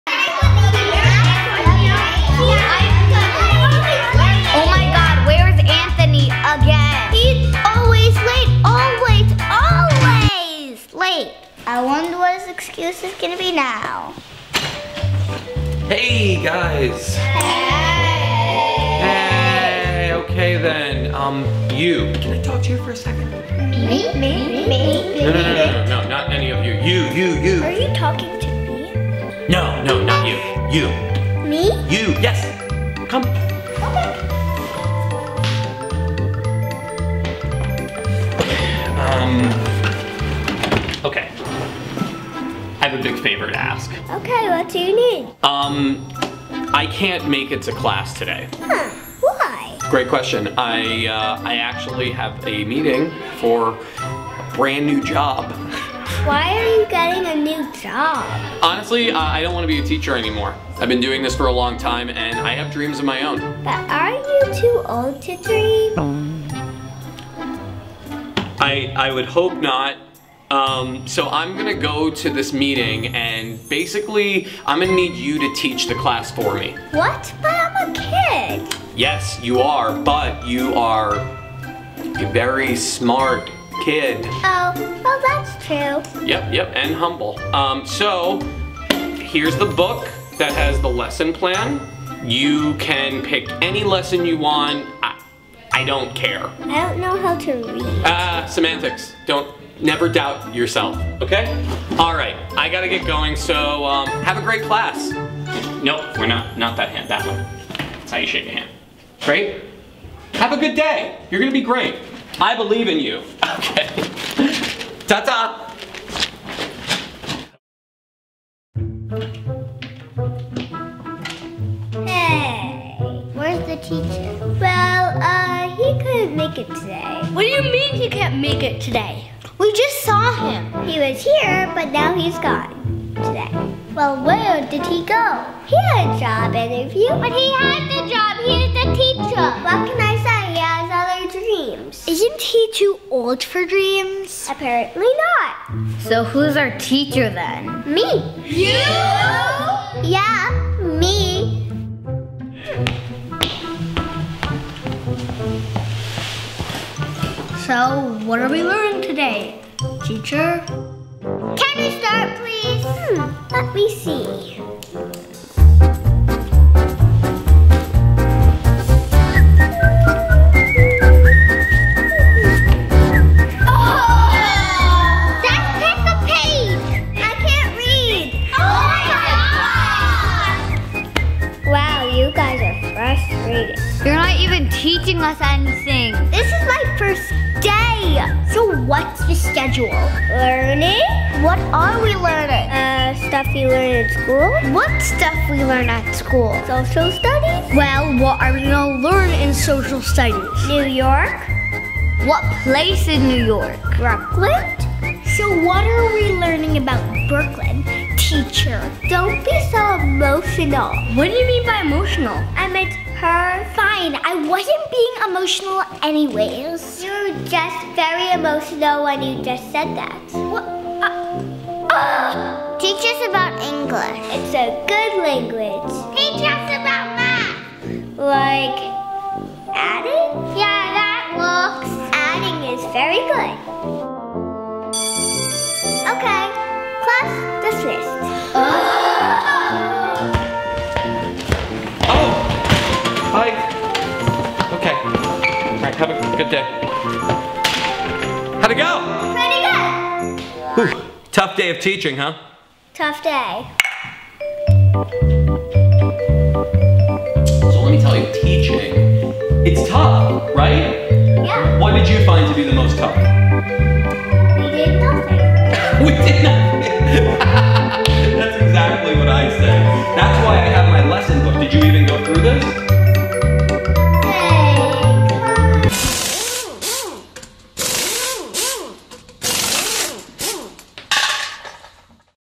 Teacher for the Day
The teacher has a job interview so this kindergartener will have to become TEACHER FOR THE DAY!